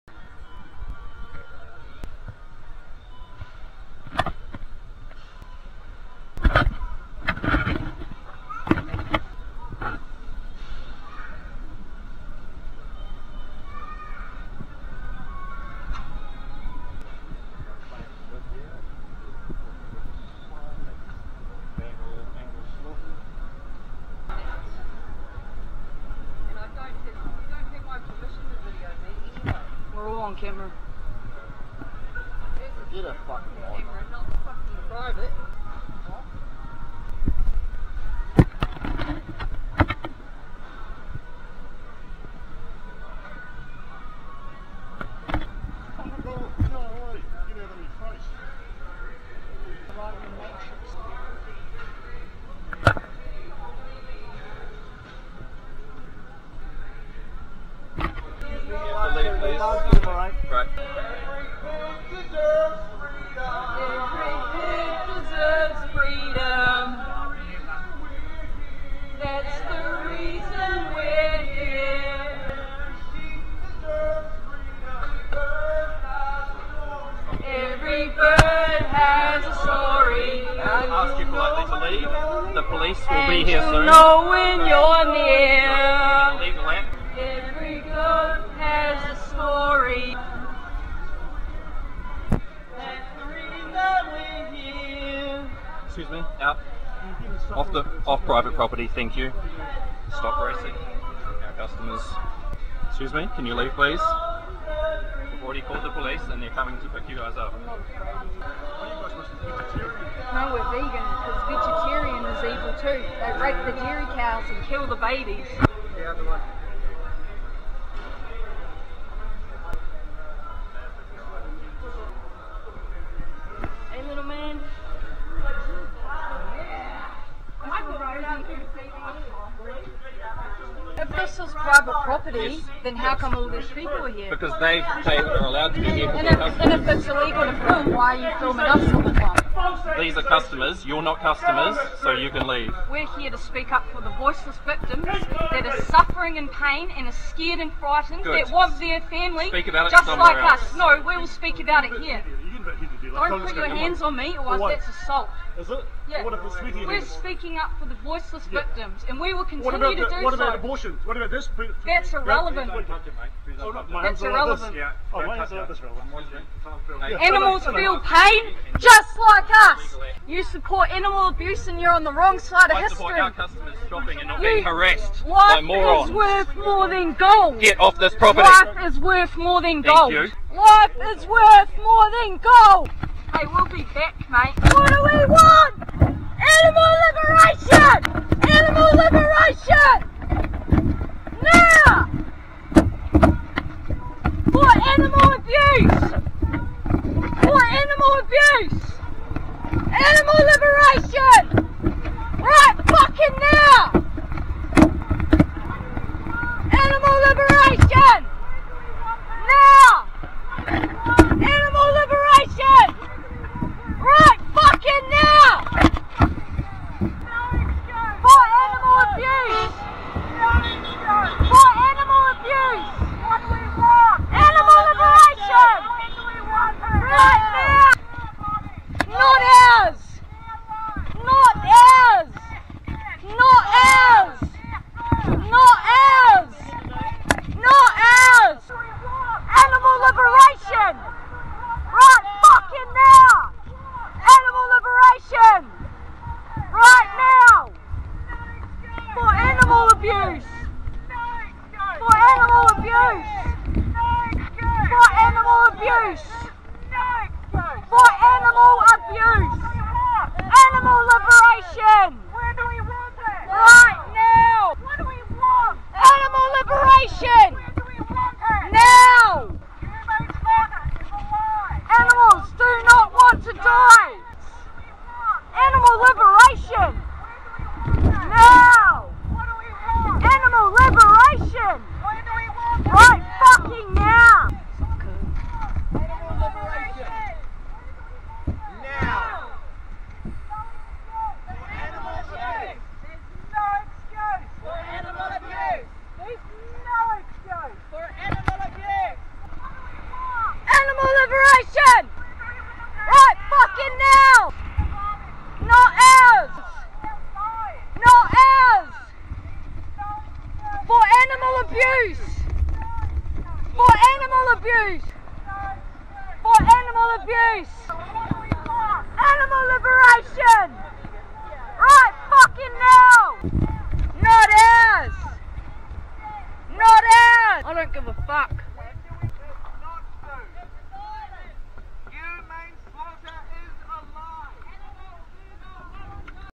Ōtautahi Vegans Disrupt Supermarket
Vegan Activists Disrupt The Human Supremacist Matrix

Zero fucks given.

FOR ABUSED ANIMALS EVERYWHERE

Film taken at PAK'nSLAVE Supermarket
Christchurch, Aotearoa